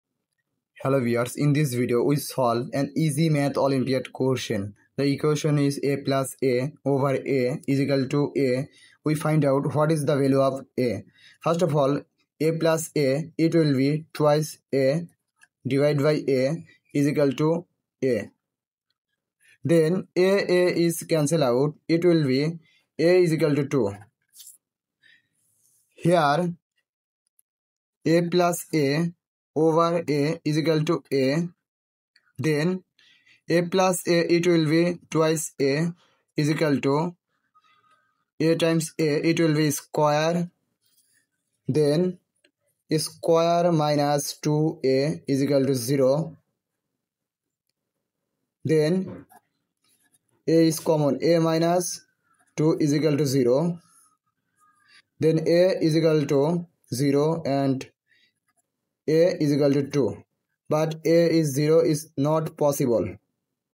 Math Olympiad question
✨ Solve this simple math puzzle: (x + x) / x = 2x 🤔
Can you find the correct value of x?
Watch till the end to see the step-by-step solution!

Do you want me to also create title + tags for this video so it ranks better?